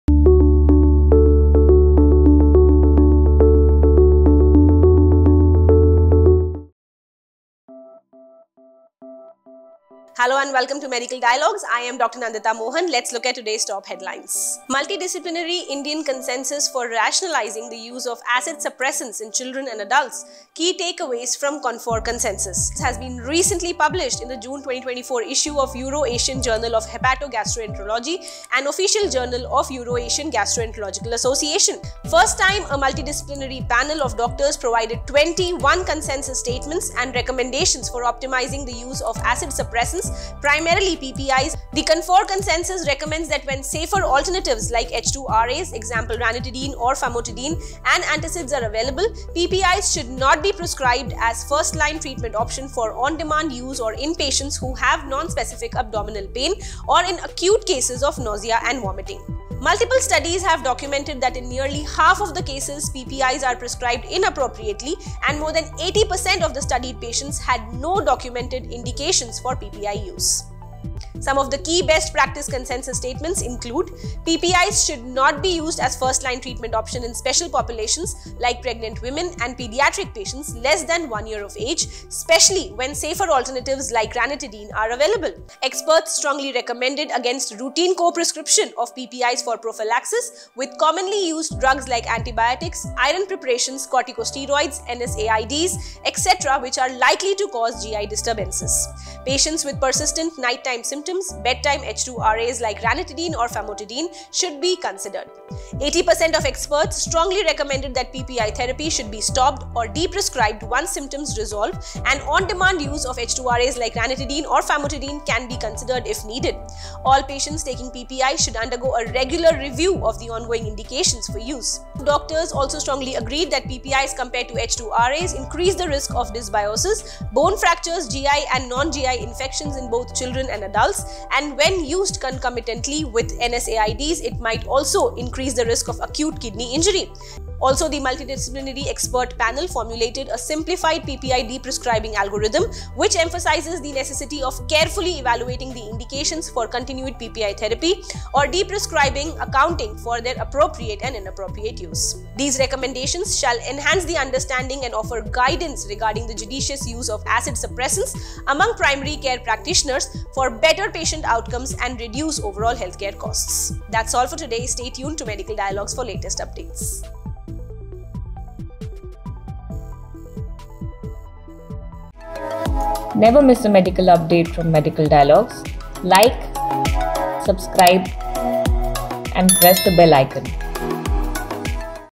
CONFOR Consensus for Rationalizing the Use of Acid Suppressants in Children & Adults
A Multidisciplinary Consensus for Rationalizing the Use of Acid Suppressants in Children and Adults: CONFOR CONSENSUS has been recently published in the June 2024 issue of Euroasian Journal of Hepato-Gastroenterology, an official journal of Euroasian Gastroenterological Association.

CONFOR is a CONsensus among multidisciplinary team (MDT) of healthcare professionals involving gastroenterologists, hepatologists, pediatric gastroenterologists, pediatricians, otolaryngologists, cardiologists, nephrologists, gynecologist, and orthopedists. 1st time a Multidisciplinary panel of doctors provided 21 consensus statements & recommendations for optimizing use of acid suppressants primarily PPIs and provided a simplified deprescribing algorithm.

The CONFOR consensus recommends that when safer alternatives like H2RAs (e.g., ranitidine, famotidine) and antacids are available; PPIs should not be prescribed as 1st line treatment option for on-demand use / in patients who have non-specific abdominal pain or in acute cases of nausea & vomiting.

Multiple studies have documented that in nearly half of the cases, PPIs are prescribed inappropriately and more than 80% of the studied patients had no documented indications for PPI use.

Using a modified Delphi process with multi-disciplinary team of experts, the consensus included statements based on evidence and clinical considerations, and these recommendations were further classified as strong, conditional, or weak.

Some of the key best practice consensus statements include:
● PPIs should not be used as 1st line treatment option in special populations like pregnant women & pediatric patients less than one year of age, especially when safer alternatives like ranitidine are available. Overuse of AST in the pediatric population, including infants, is a growing concern which warrants careful consideration of its use in this special population.
● Experts strongly recommended against routine co-prescription of PPIs for prophylaxis with commonly used drugs like antibiotics, iron preparations, corticosteroids, NSAIDs etc., which are likely to cause GI disturbances.
● Patients with persistent night-time symptoms, bedtime H2RAs like ranitidine or famotidine should be considered.
● 80% of experts strongly recommended that PPI therapy should be stopped / deprescribed once symptoms resolve and On-demand use of H2RAs like ranitidine or famotidine can be considered, if needed. All patients taking PPI should undergo a regular review of the ongoing indications for use.
● The MDT of doctors also strongly agreed that PPIs, compared to H2RAs, increase the risk of dysbiosis, bone fractures, GI, and non-GI infections in both children and adults & when used concomitantly with NSAIDs it might also increase the risk of Acute kidney injury.

PPIs should only be prescribed if clearly indicated to minimize the associated adverse effects and complications. Also, the multidisciplinary expert panel formulated a simplified PPI deprescribing algorithm which emphasizes the necessity of carefully evaluating the indications for continued PPI therapy or deprescribing, accounting for their appropriate and inappropriate use.

These recommendations shall enhance the understanding and offer guidance regarding the judicious use of acid suppressants amongst primary care practitioners for better patient outcomes and reduce overall healthcare costs.